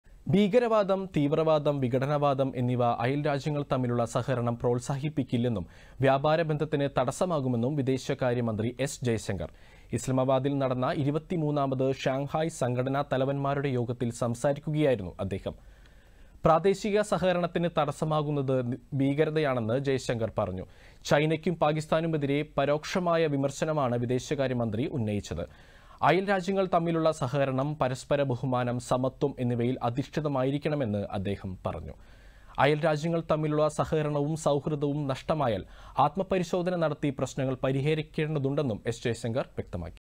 "ഭീകരവാദം, തീവ്രവാദം, വിഘടനവാദം എന്നിവ അയൽരാജ്യങ്ങൾ തമ്മിലുള്ള സഹകരണം പ്രോത്സാഹിപ്പിക്കില്ല"
ഭീകരവാദം, തീവ്രവാദം, വിഘടനവാദം എന്നിവ അയൽരാജ്യങ്ങൾ തമ്മിലുള്ള സഹകരണം പ്രോത്സാഹിപ്പിക്കില്ലെന്നും വ്യാപാര ബന്ധത്തിന് തടസ്സമാകുമെന്നും വിദേശകാര്യമന്ത്രി എസ്.ജയ്ശങ്കർ.

ഇസ്ലാമാബാദിൽ നടന്ന 23-ാമത് ഷാങ്ഹായ് സംഘടനാ തലവൻമാരുടെ യോഗത്തിൽ സംസാരിക്കുകയായിരുന്നു അദ്ദേഹം.

പ്രാദേശിക സഹകരണത്തിന് തടസമാകുന്നത് ഭീകരതയാണെന്ന് ജയ് ശങ്കർ പറഞ്ഞു.

ചൈനയ്ക്കും പാക്കിസ്ഥാനുമെതിരെ പരോക്ഷമായ വിമർശനമാണ് വിദേശകാര്യമന്ത്രി ഉന്നയിച്ചത്.

അയൽ രാജ്യങ്ങൾ തമ്മിലുള്ള സഹകരണം /പരസ്പര ബഹുമാനം , സമത്വം എന്നിവയിൽ അധിഷ്ഠിതമായിരിക്കണമെന്ന് അദ്ദേഹം പറഞ്ഞു. .

അയൽ രാജ്യങ്ങൾ തമ്മിലുള്ള സഹകരണവും സൗഹൃദവും നഷ്ടമായാൽ ആത്മപരിശോധന നടത്തി പ്രശ്നങ്ങൾ പരിഹരിക്കേണ്ടതുണ്ടെന്നും എസ്.ജയശങ്കർ വ്യക്തമാക്കി.